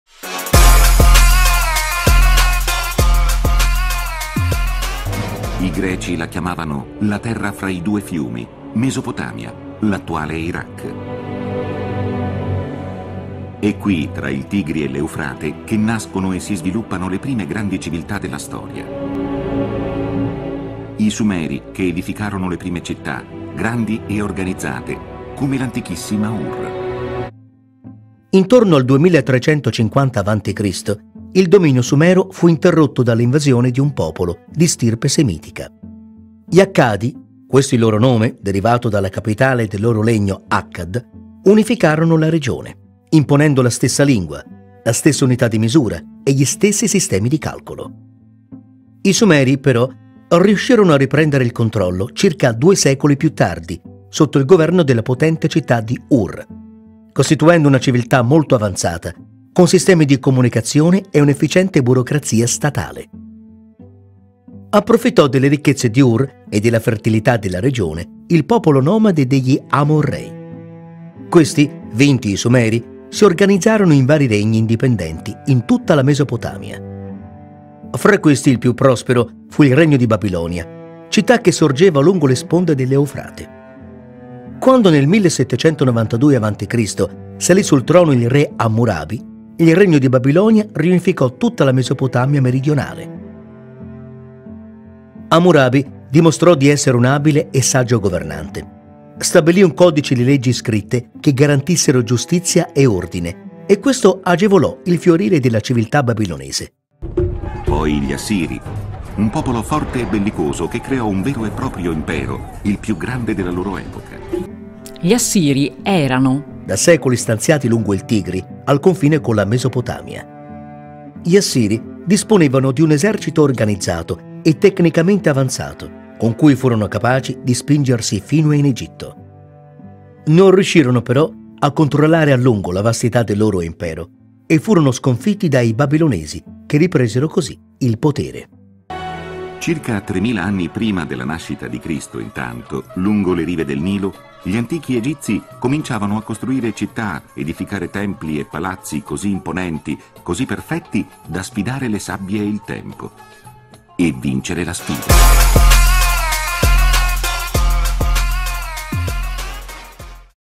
Le antiche civiltà fluviali (prima parte)
Presentazione generale delle antiche civiltà che si svilupparono nel territorio della Mezzaluna fertile dall'Egitto all'Iraq